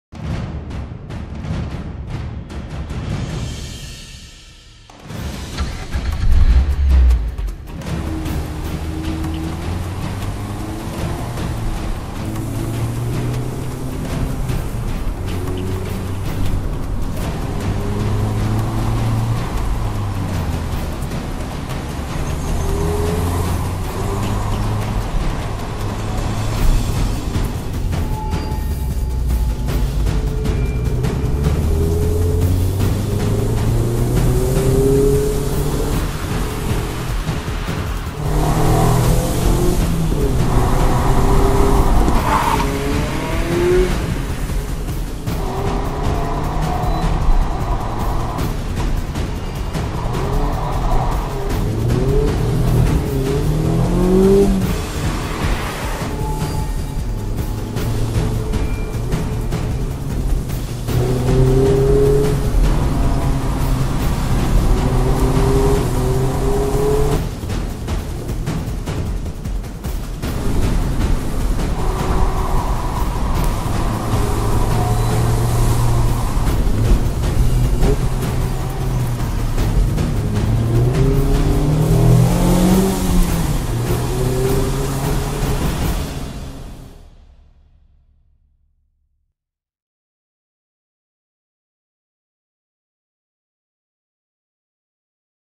Courtenay Mazda: 2016 Mazda MX-5 Miata VS The Mountains: Col De Turini on Vancouver Island
Immerse yourself in the 2016 Mazda MX-5 Miata as it takes on the 34 hairpins of France’s Col De Turini